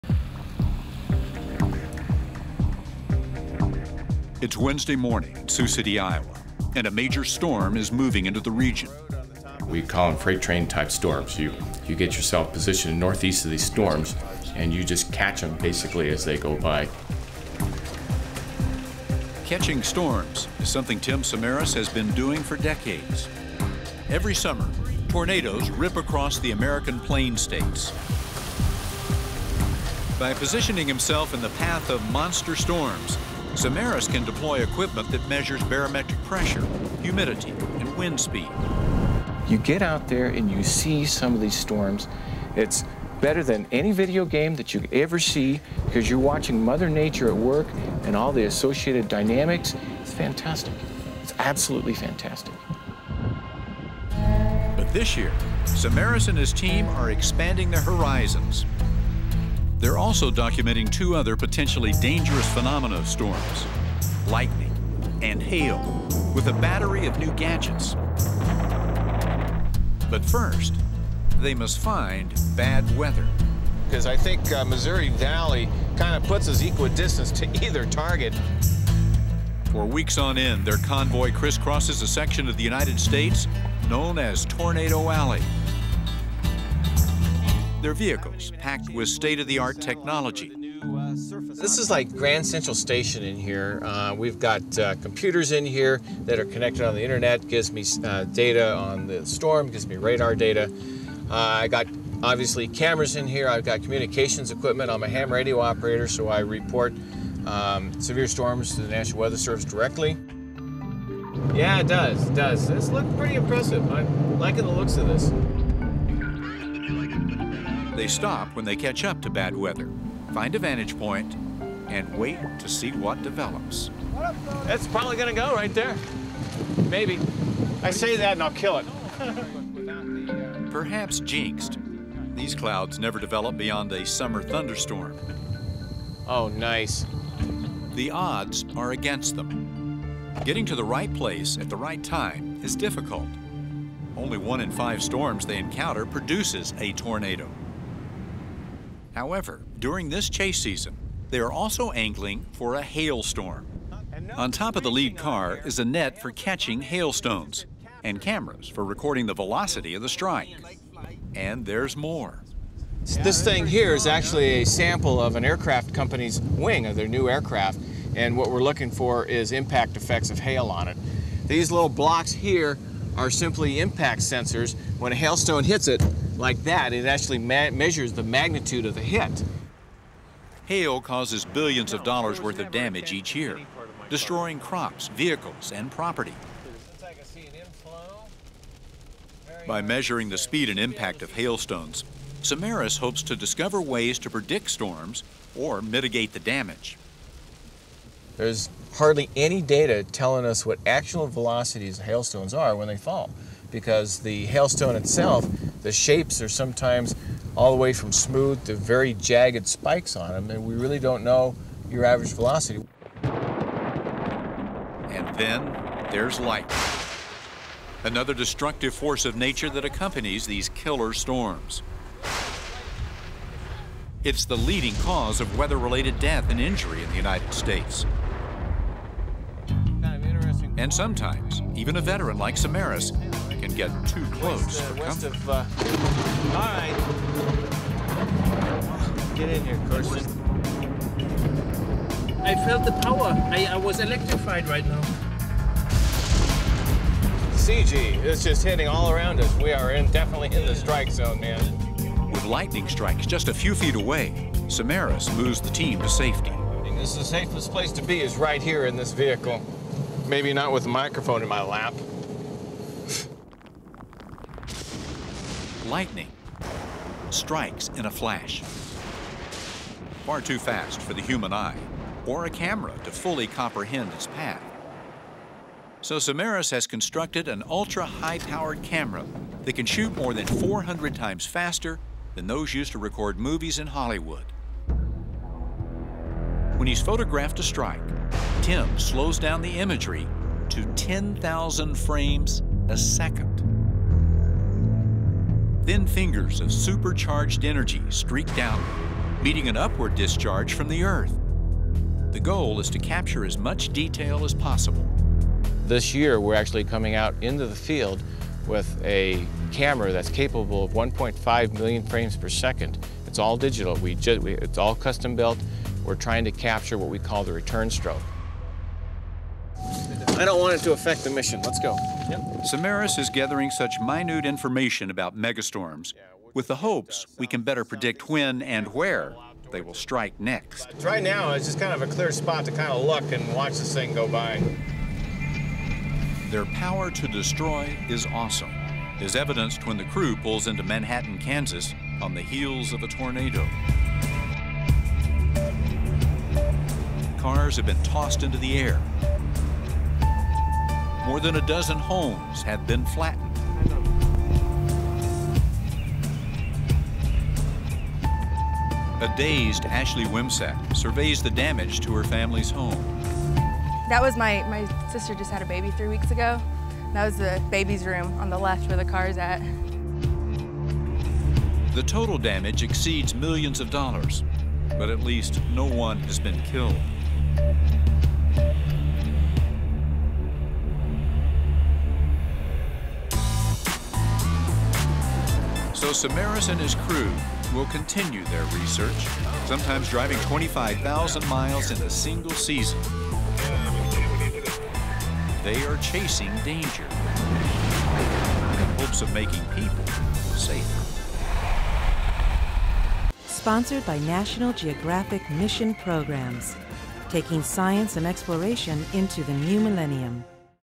Tornadoes, Lightning in Rare Video | National Geographic
As tornado season touches down in the U.S., see a high-tech tornado chaser's stunning footage of twisters, and of lightning as you've never seen it - in super slow-motion!